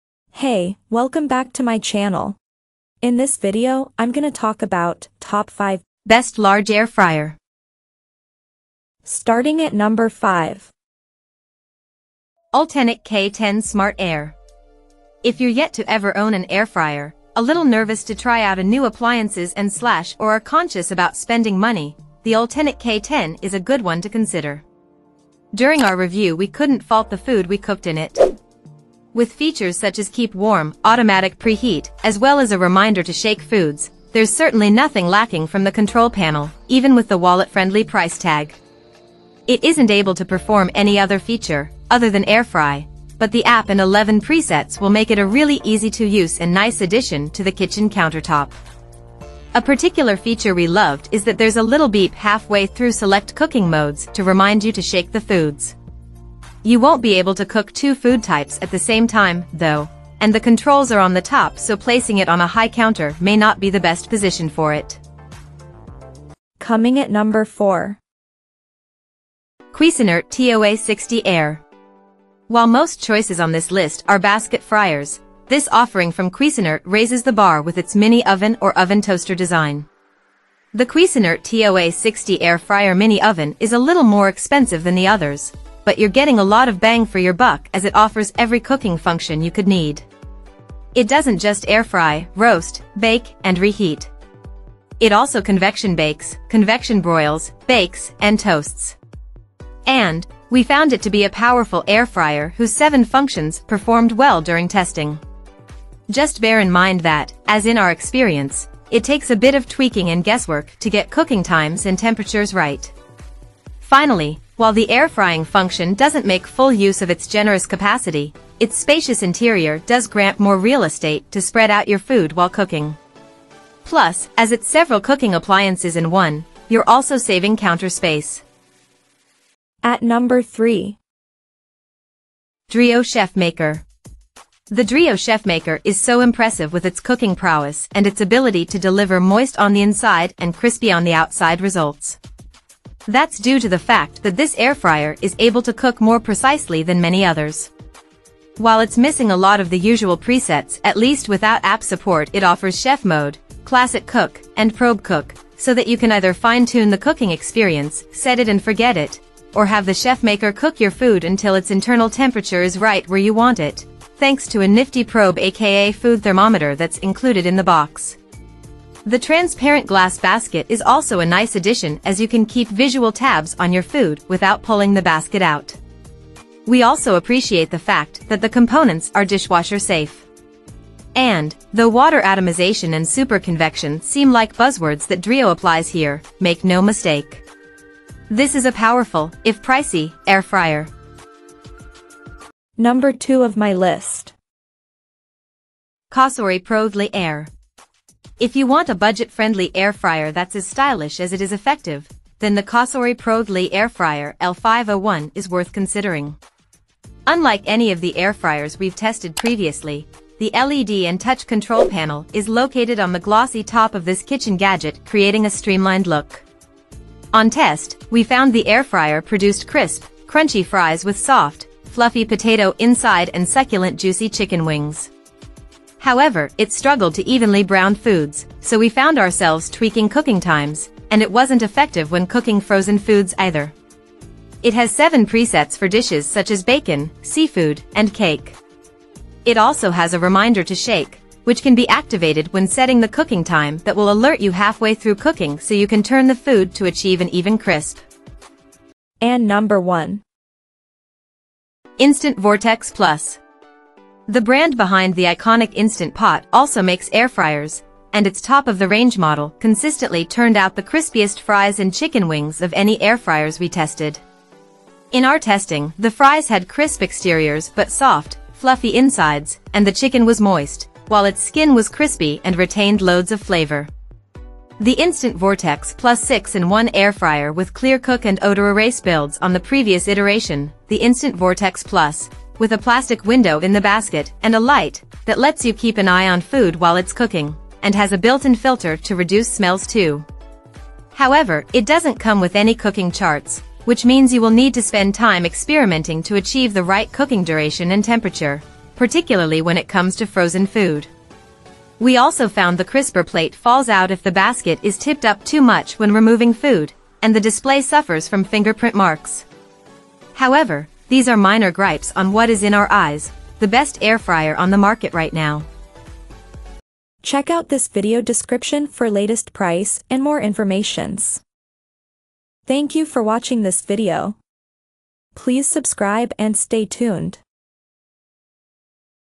Top 5 Best Large Air Fryer in 2024
Here is the Updated Prices of the Best Large Air Fryer  in 2023!

Things that we mentioned in this video:
1. Instant Vortex Plus - B096N3FTZP
2. Cosori Pro LE Air - B0936FGLQS
3. Dreo Chefmaker - B0C5JHKGFP
4. Cuisinart TOA-60 Air  - B01K0W8LTE
5. Ultenic K10 Smart Air  - B09CYYNTKK